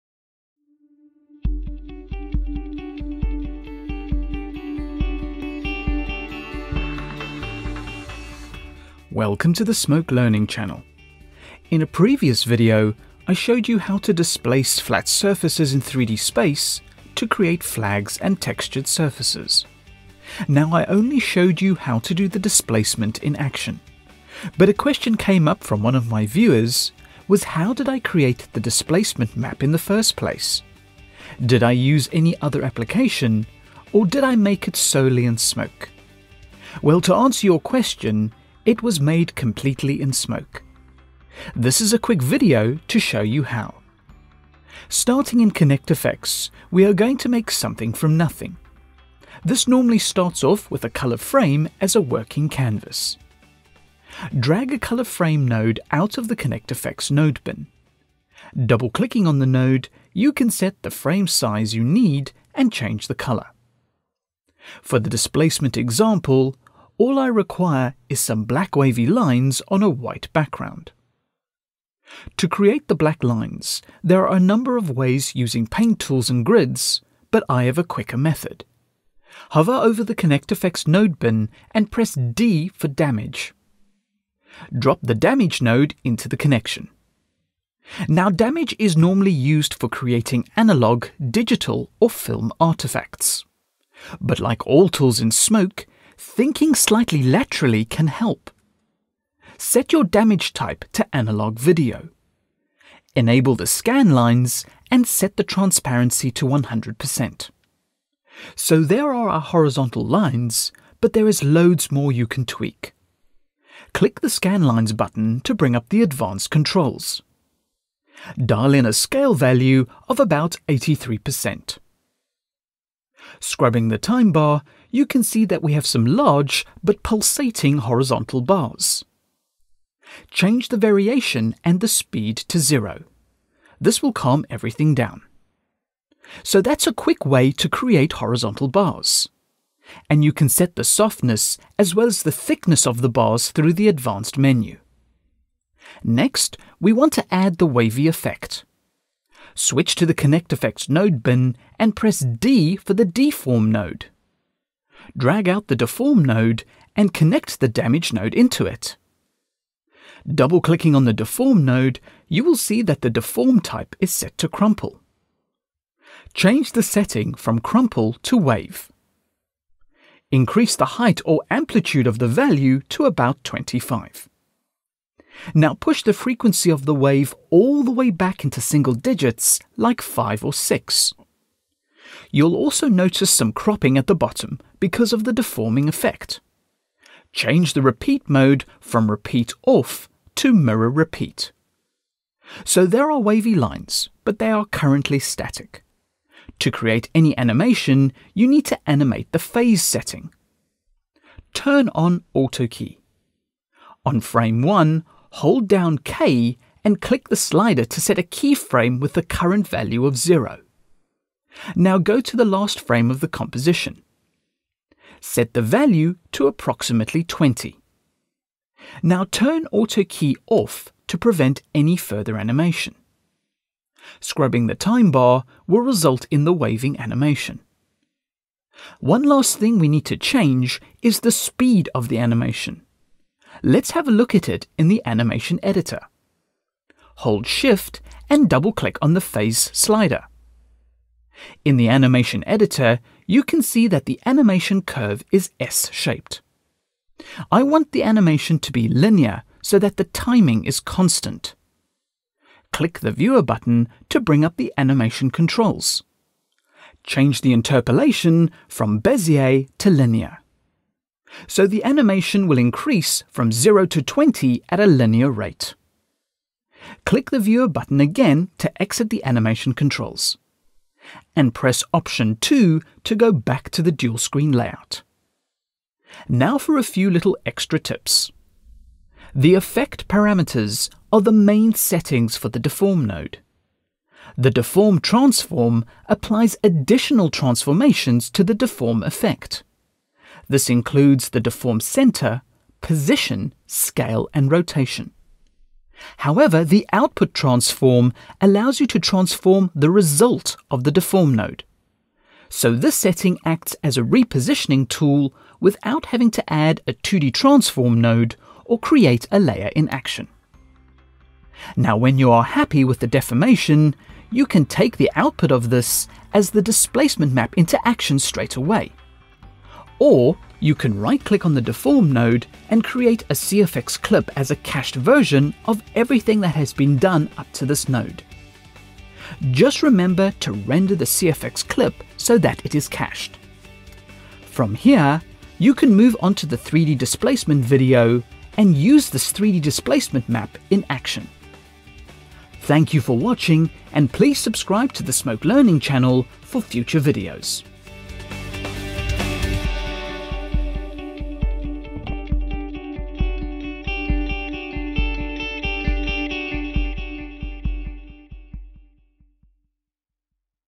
3D Displacement: Creating a Displacement Map
Level: Beginner

In response to a user request, this video teaches you how to create a displacement map in ConnectFX to be used  for the 3D displacement in Action.